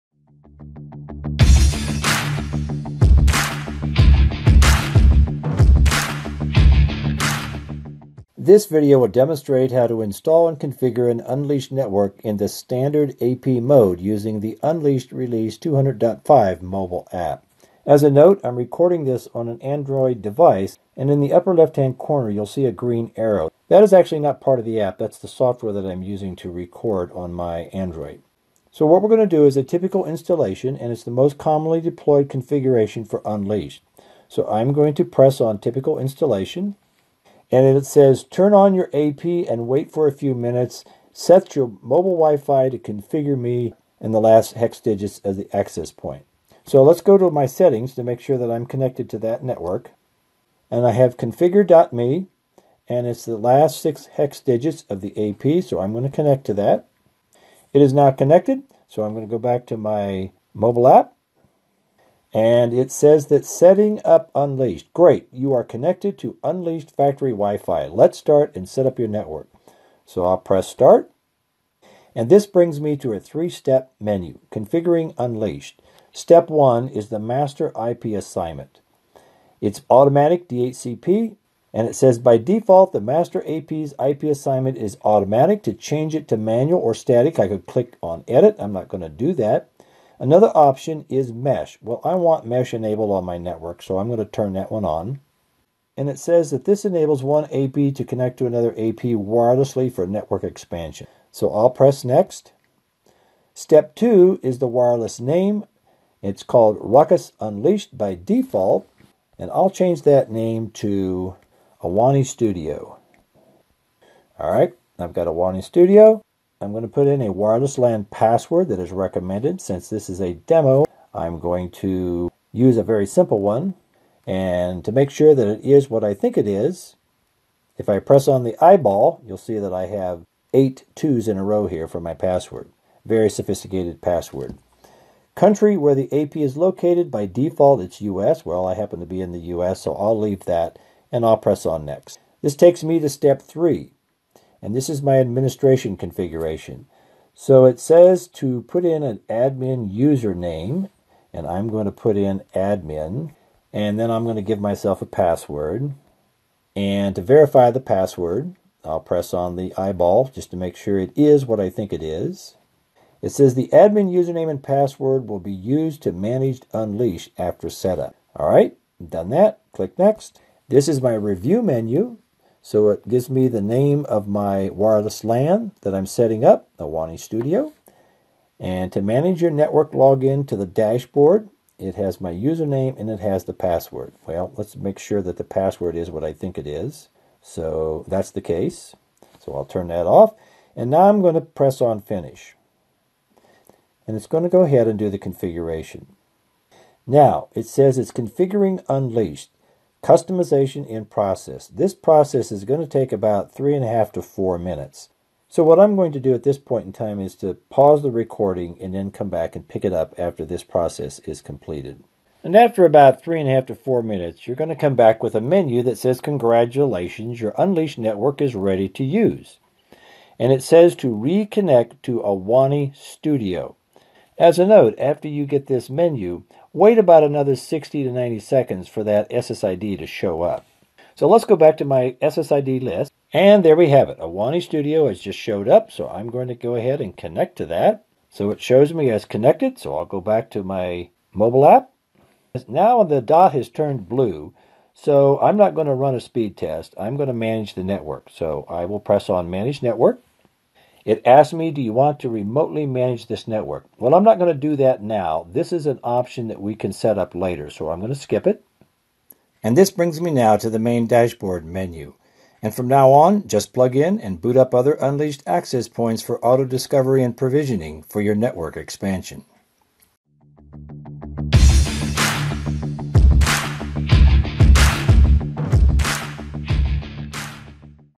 Unleashed Standard AP Installation with Mobile App
This video demonstrates using Unleashed 200.5 for a standard AP Installation using the mobile app. (Release 200.5)